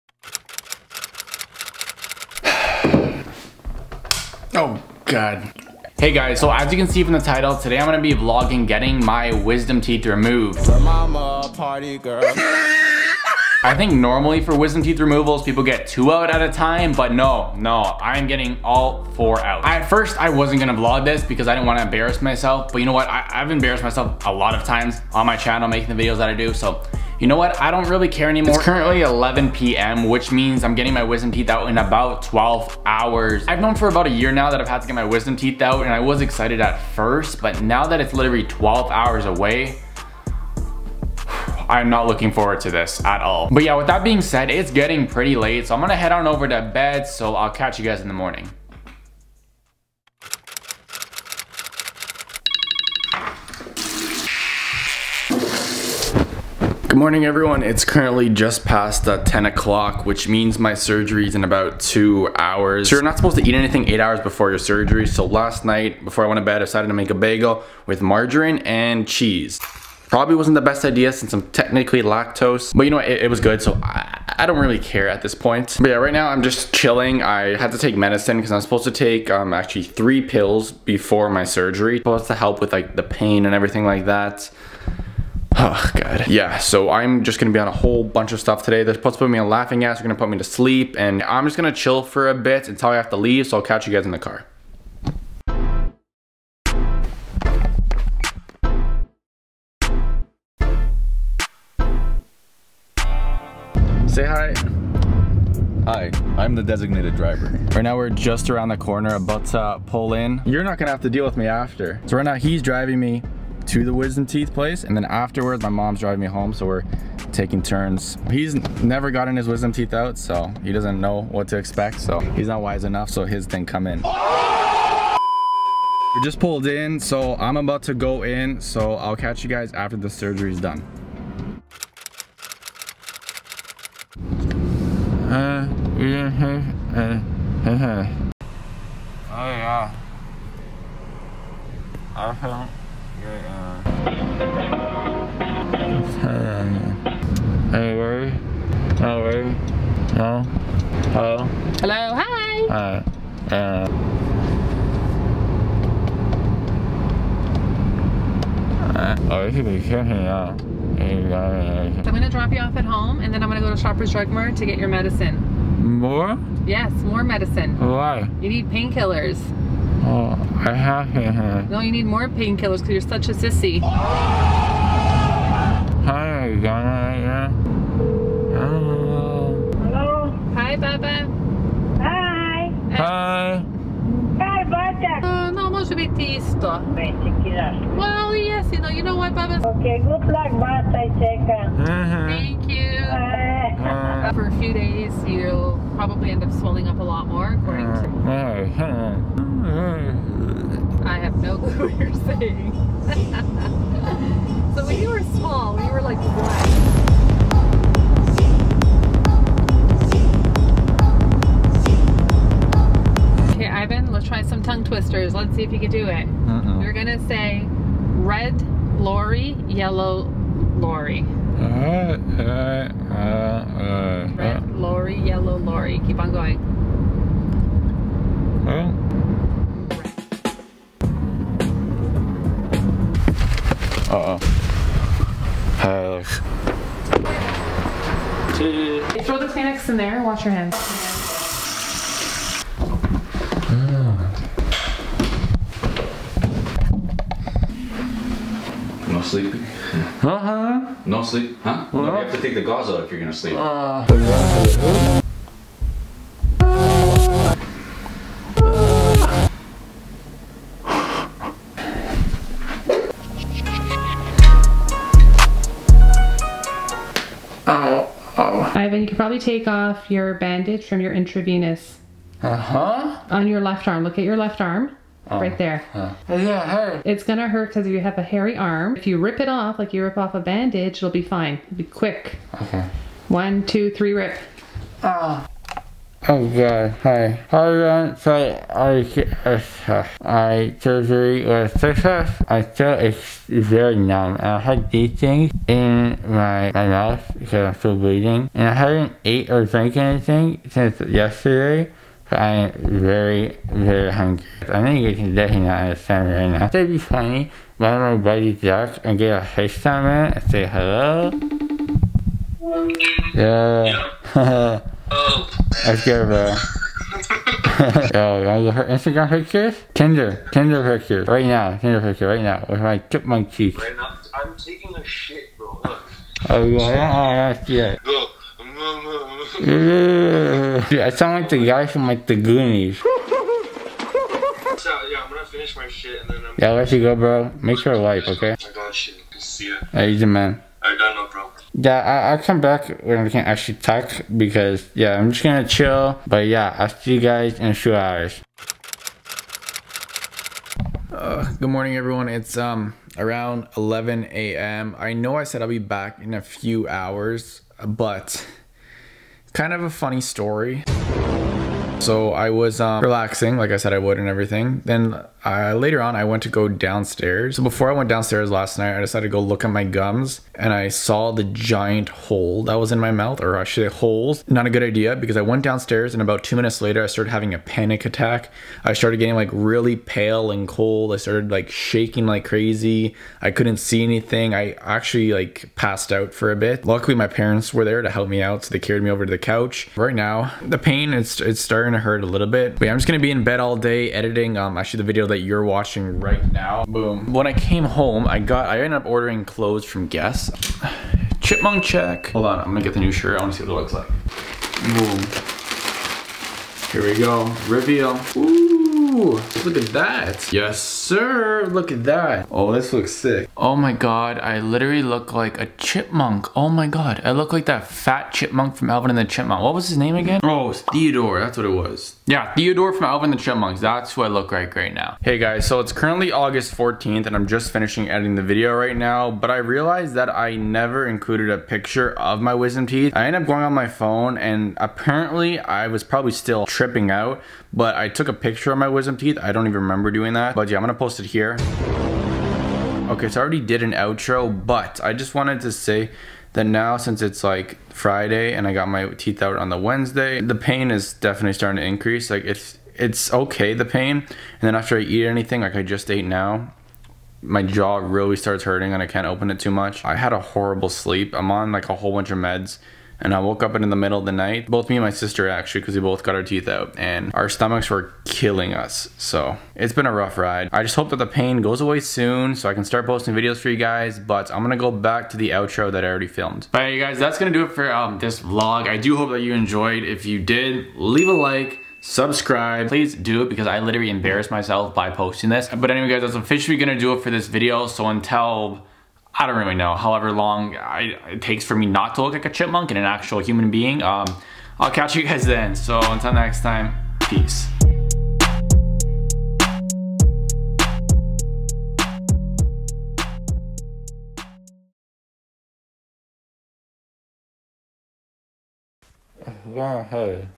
i got my WISDOM TEETH removed (vlog)
Hey everyone! In today's video I am vlogging my experience with getting all four of my wisdom teeth removed! In this video I vlog my funny wisdom teeth removal and the aftermath of getting all my wisdom teeth out! this was an experience I’ll never forget.